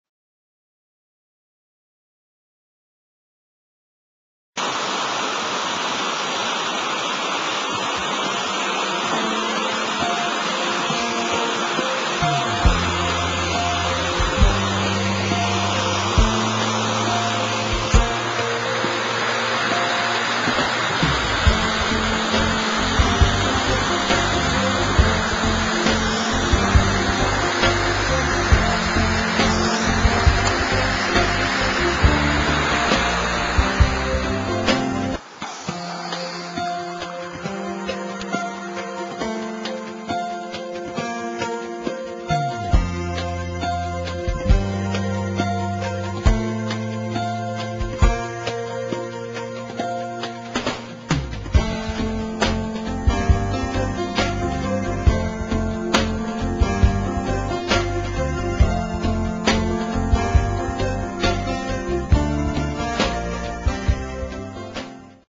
The Mt Buffalo Gorge
A visit to the Mt Buffalo Gorge, Mt Buffalo National Park, 320 km NE of Melbourne, on April 25 2008.